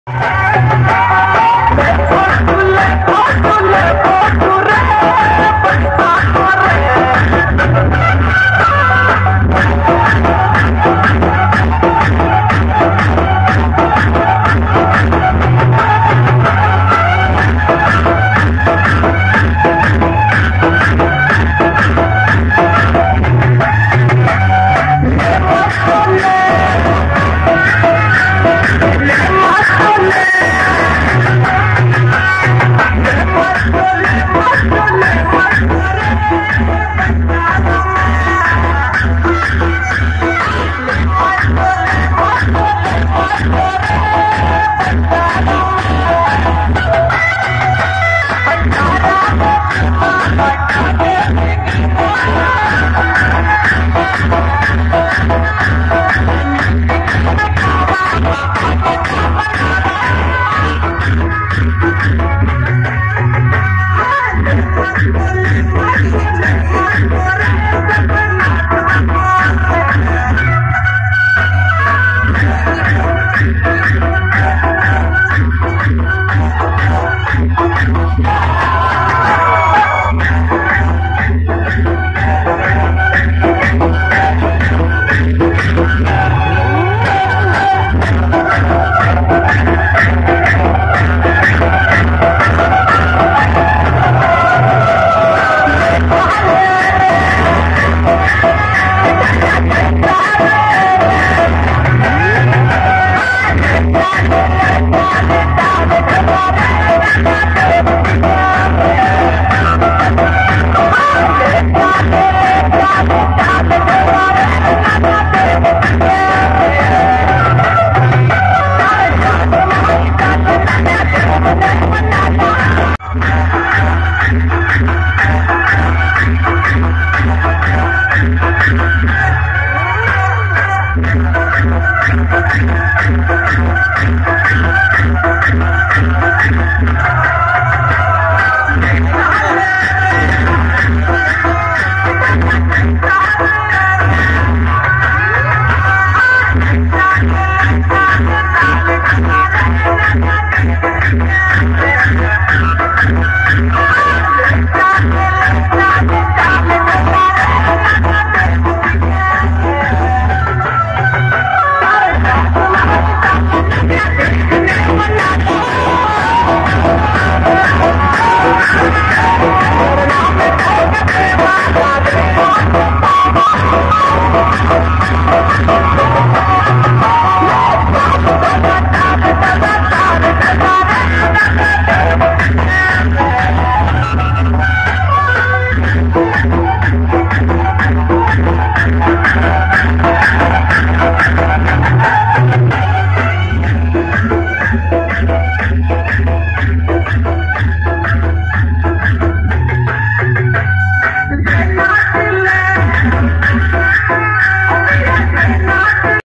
😱🔥ভোলাবাবা সাউন্ড🔥😱 বিরাটি তে শিবের রাত গাজন উপলক্ষে 🙏 বাজাচ্ছে।।
প্লিজ 🥺 🙏🏻.
লাইক 👍.
কমেন্ট ✍️.
সেয়ার 👉.